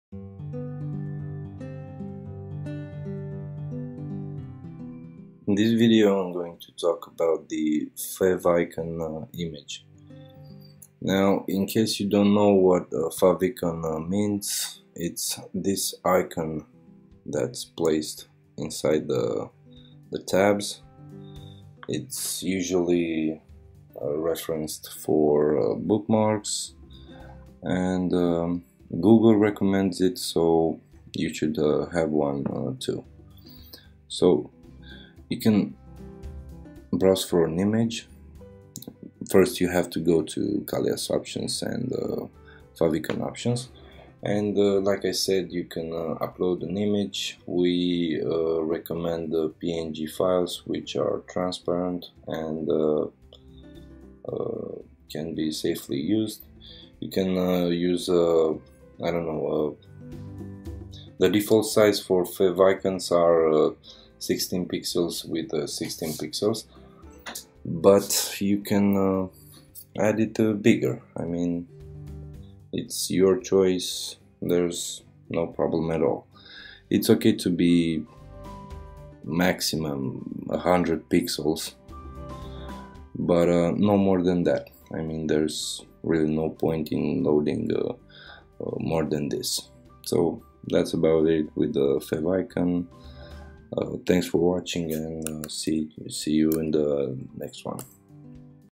Favicon option in Kallyas options ( Kallyas WordPress theme v4.0 )
This screencast should explain what the "Favicon options" section in Kallyas options does, what the actual option-fields do and how to use and tweak them.
This video is based on Kallyas WordPress theme v4.0 series.